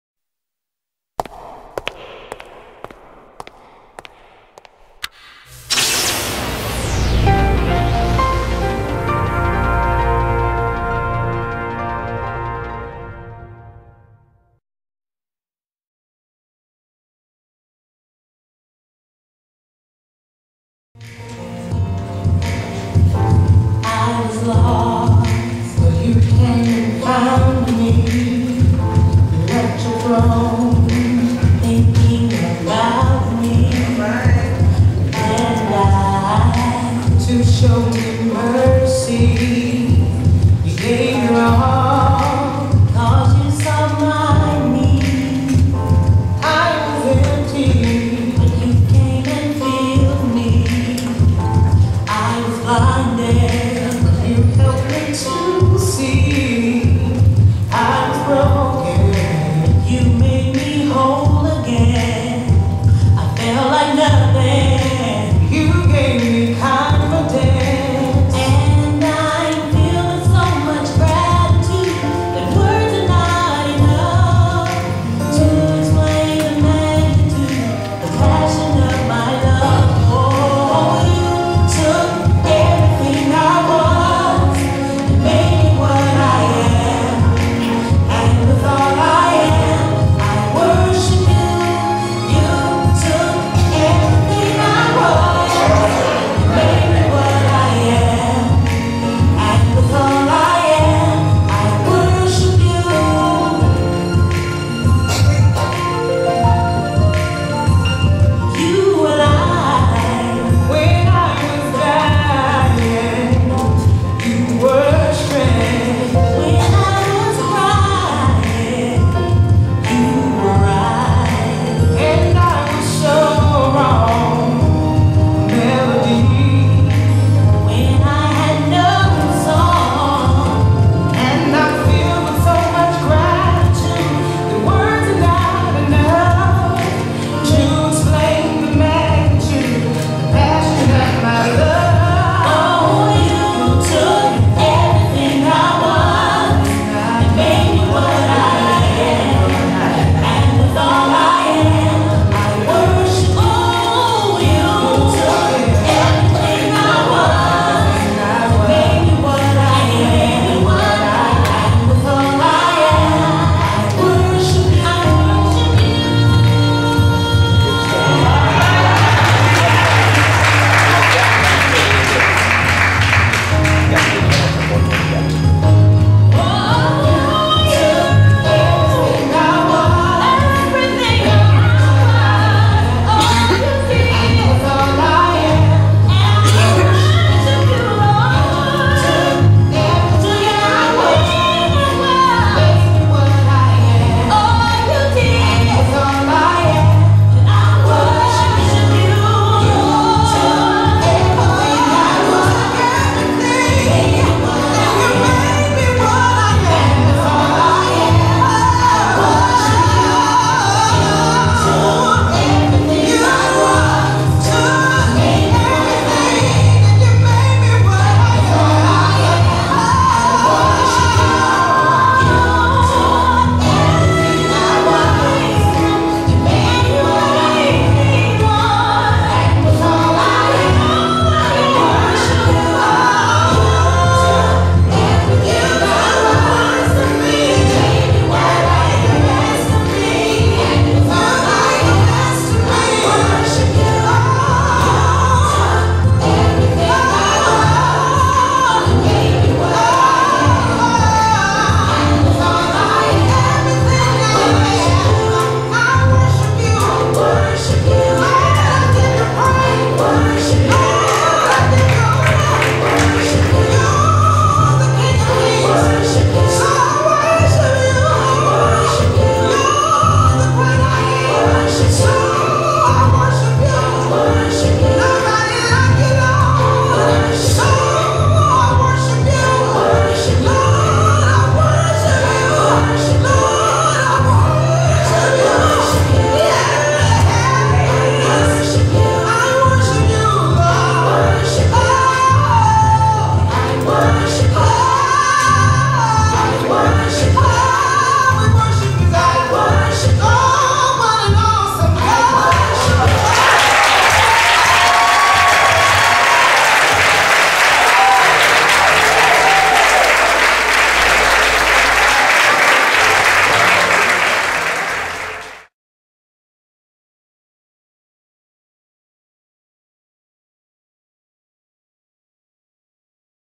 Pastors Praise Program - Light of the World (1)
5th Annual Pastors' Praise in Worship and Psalms program for the Waycross MLK Holiday Celebration, Light of the World Ministries 1st Performance